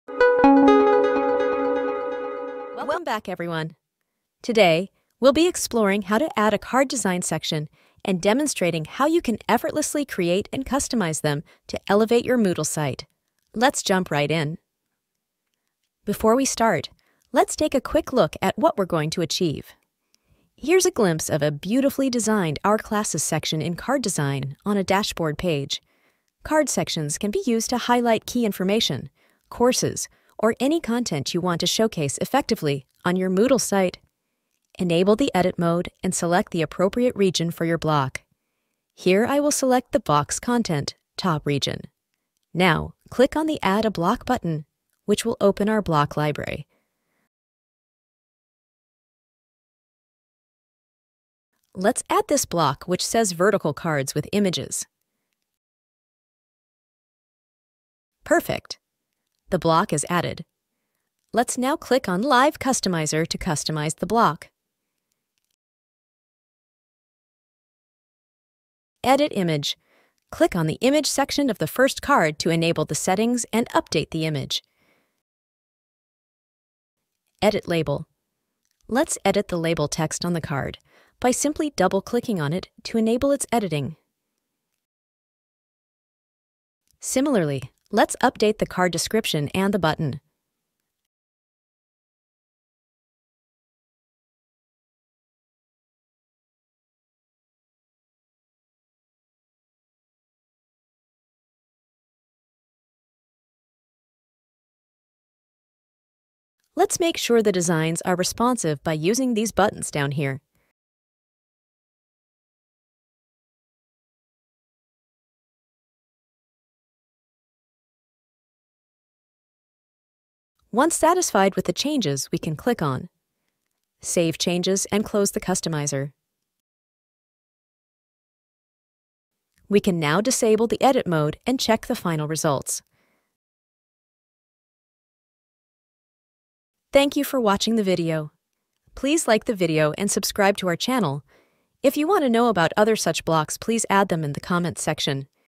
How to design Card Layouts in Moodle?
How to design Card Layouts in Moodle? Today, we'll be exploring how to add a card design section and demonstrating how you can effortlessly create and customize them to elevate your Moodle site.

Timestamps -
00:00 - Intro
00:13 - Lets get it started with Edwiser Page Builder
00:32 - How to add Contact Us Scetion?
00:37 - Full width top section in Moodle
00:50 - Added the block
00:54 - Live Customizer
01:12 - How to edit text?
01:30 - Done